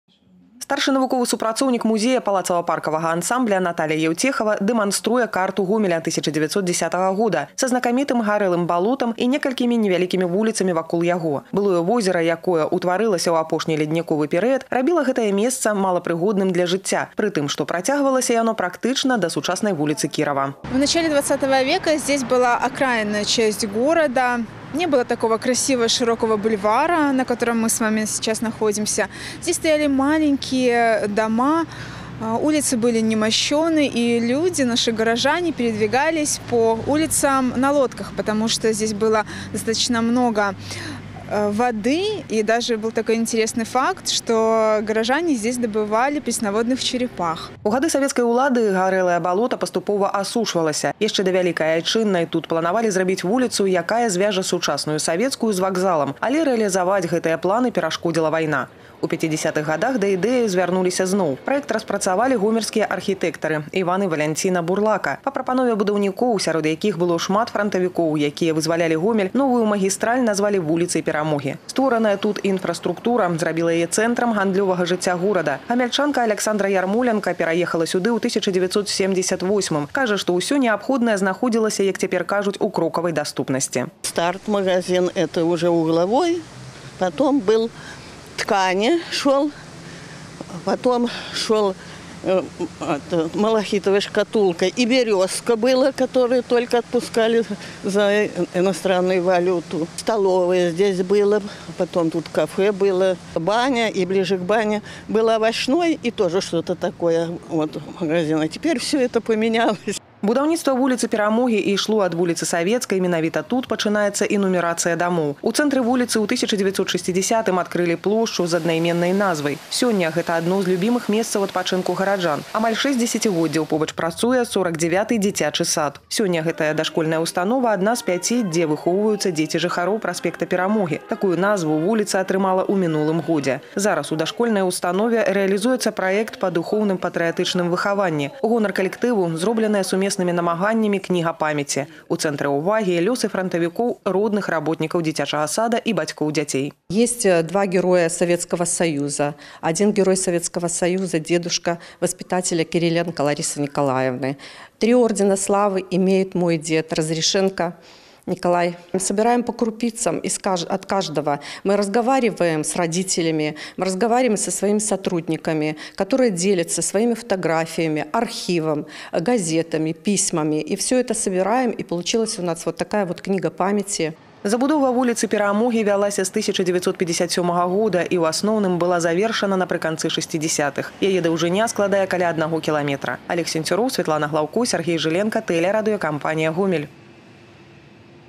Было горелое болото, в котором добывали черепах. Вспоминаем историю гомельского пр. Победы
Сегодня эту улицу называют «оазисом в центре Гомеля».

В канун 80-летия освобождения Беларуси от немецко-фашистских захватчиков историю проспекта Победы вспомнили наши корреспонденты.